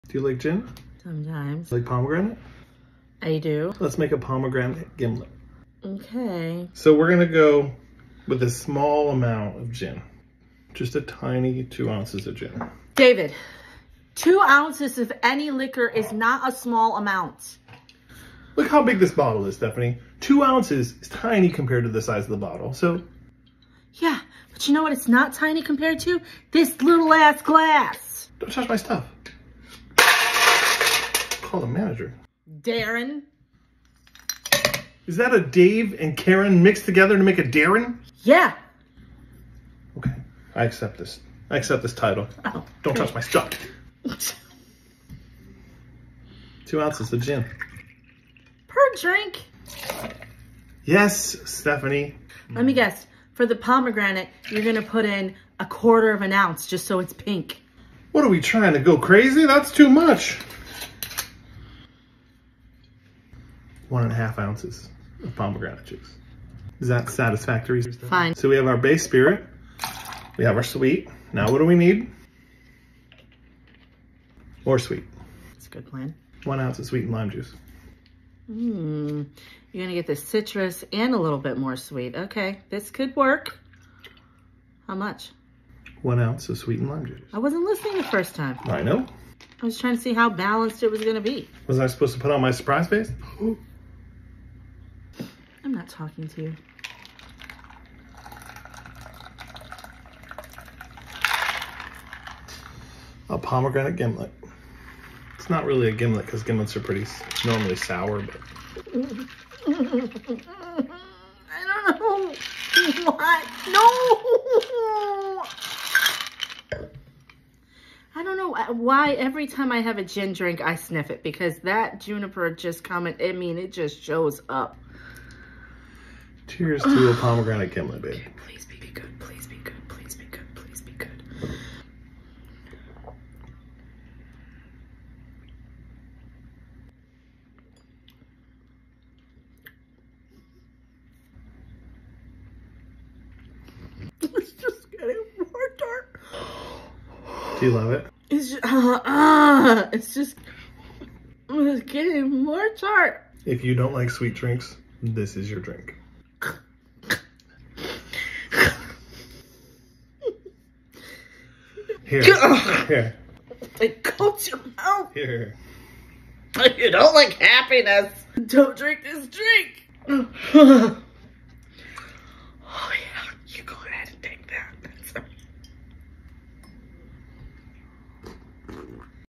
Pomegranate Gimlet- A tart upgrade
Join us in this video as we craft the elegant Pomegranate Gimlet.
We'll guide you through each step, from selecting the perfect gin to the final shake and pour. Discover the secrets to achieving the perfect balance of botanicals and sweet-tart flavor in every glass.